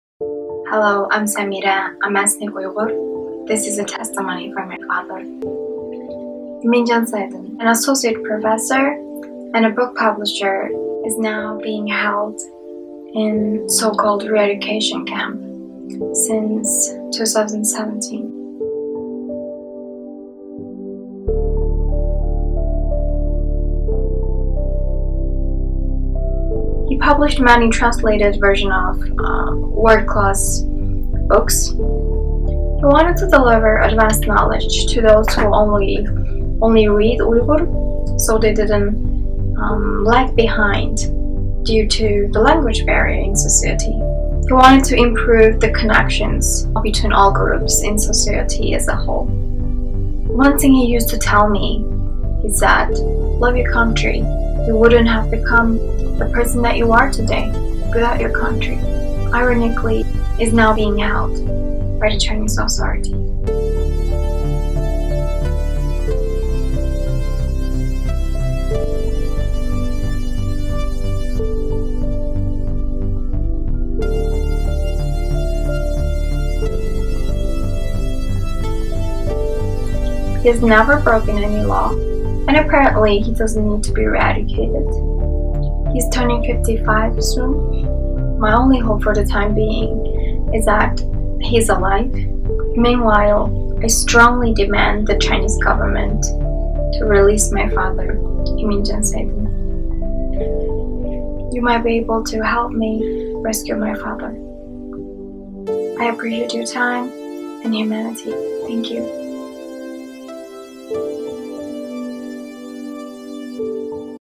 Beat 420: Semire, USA (English, 中文)
Semire's testimony for her father, Iminjan Seydin with Chinese text. A renowned publisher, Iminjan was recently given a 15-year sentence.